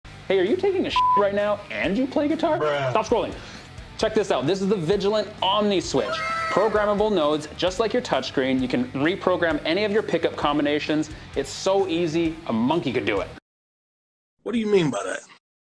REWIRE YOUR GUITAR EASILY? NO Soldering Iron??? | Vigilant Omni Switch Teaser
A short clip about the imminently approaching Vigilant Guitars OMNI SWITCH! A revolution in guitar design, this system allows you to completely reconfigure your pickups without having to so much as take off a back plate or touch a tool. Simply connect your Vigilant Omni Switch equipped guitar to your computer or smart device, and fire up the pickup configurator page from our website. This is exceptionally useful for Fishman Fluence pickups, but works just fine with passive pickups as well.

Take care and STAY VIGILANT.